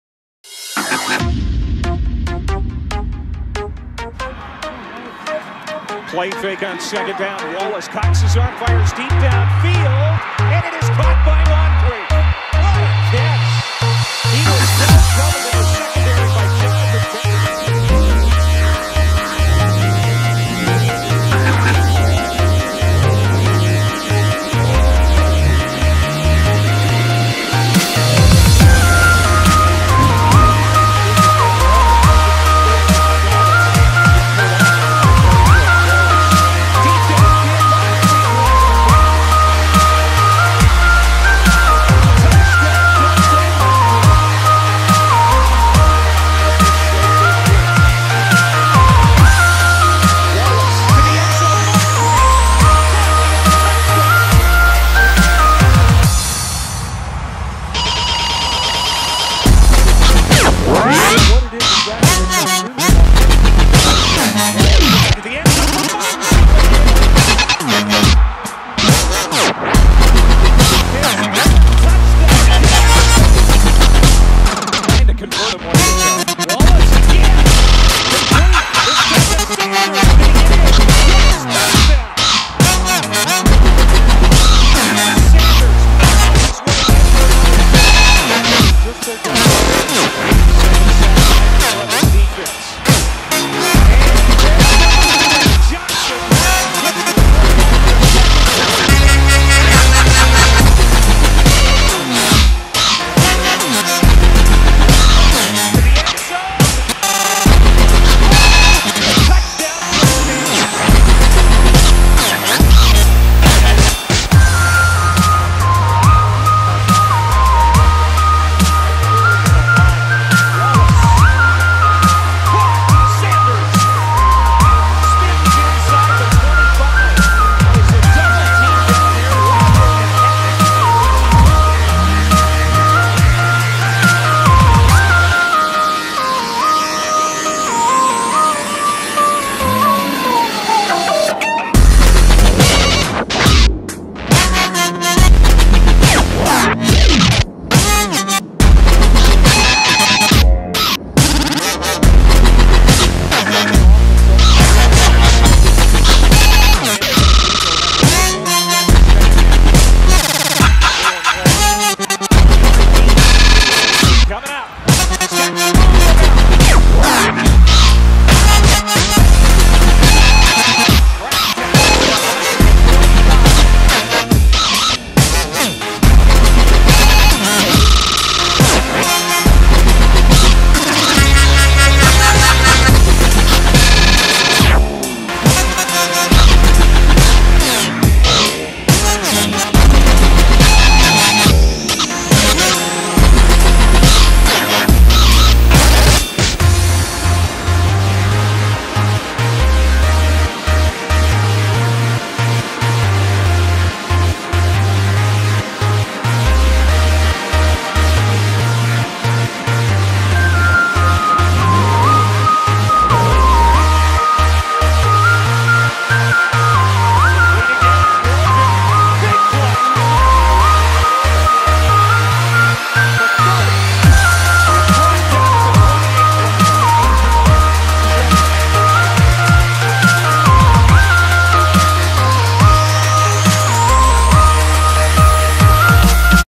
Ole miss football
OLE MISS!!! Ole Miss football highlights playing Miss st. and Vanderbilt